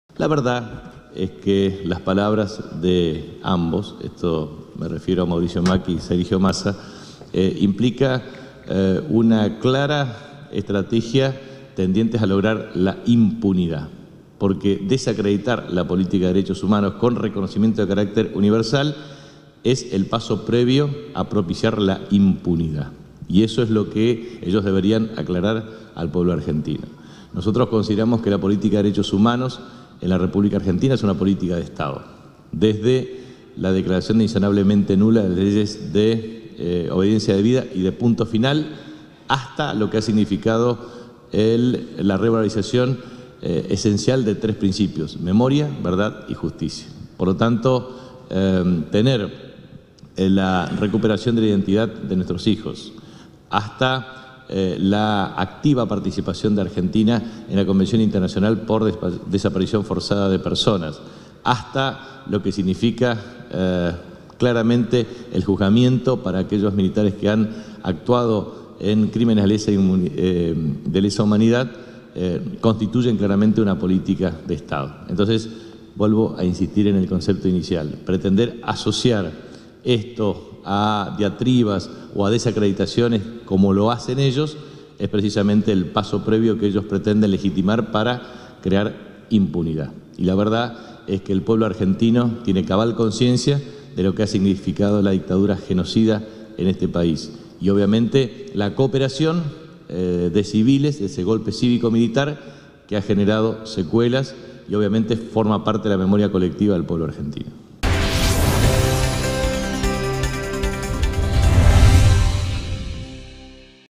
Capitanich: "Desacreditar la política de Derechos Humanos es una estrategia para la impunidad"
El jefe de Gabinete, Jorge Capitanich, aseguró hoy que tanto el líder del Frente Renovador, Sergio Massa, como el jefe de Gobierno porteño, Mauricio Macri, al "desacreditar la política de derechos humanos" esconden una estrategia tendiente a "lograr la impunidad" de los responsables de delitos de lesa humanidad cometidos durante la última dictadura.

"Las palabras de ambos, de Massa y de Macri, implican una clara estrategia tendiente a lograr la impunidad porque desacreditar la política de derechos humanos, con reconocimiento de carácter universal, es el paso previo a propiciar la impunidad y eso es lo que ellos deberían aclarar al pueblo argentino", planteó el jefe de ministros.

Así lo hizo esta mañana, en el marco de su habitual conferencia de prensa en la Casa de Gobierno, en la que subrayó que "la política de derechos humanos en la Argentina es una política de Estado".

"Tener la recuperación de la identidad de nuestros hijos hasta la activa participación de Argentina en la Convención Internacional por Desaparición Forzada de Personas hasta lo que significa claramente el juzgamiento para aquellos militares que han actuado en crímenes de lesa humanidad constituyen claramente una política de Estado", postuló el jefe de ministros.

En este marco, afirmó que, "pretender asociar esto a diatribas o desacreditaciones como lo hacen ellos, es el paso previo que ellos pretenden legitimar para propiciar la impunidad", aunque remarcó que "el pueblo argentino tiene cabal conciencia de lo que ha significado la dictadura genocida en este país".

En una entrevista exclusiva publicada ayer en el diario El Tribuno, de Salta, Massa postuló que "la Argentina tiene que cerrar la etapa de derechos humanos", aunque indicó que no se puede "dejar de mirar que en el país hubo treinta mil desaparecidos".

En tanto, días atrás, el Jefe de Gobierno porteño y precandidato presidencial por el Pro, Mauricio Macri, había señalado que, en caso de acceder a la presidencia, terminaría con lo que llamó "el curro de los derechos humanos".